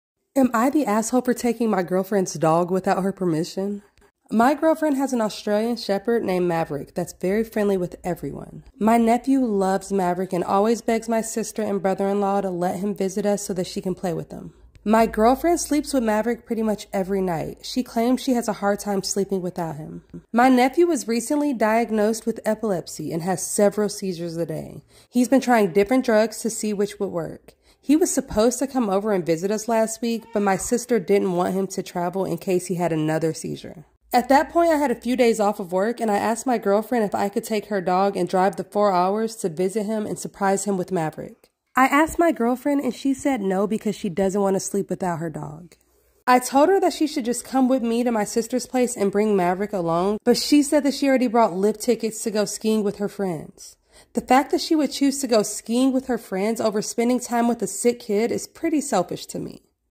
Dang 😬 Who's TA here?? - Resin Art Storytime Tutorial - Gorgeous Zodiac Hair Picks
Full story on Reddit!! 📖 Sorry can't find the link!! 😭

If you are interested in purchasing my resin creations check out my Etsy shop! 💳🛍️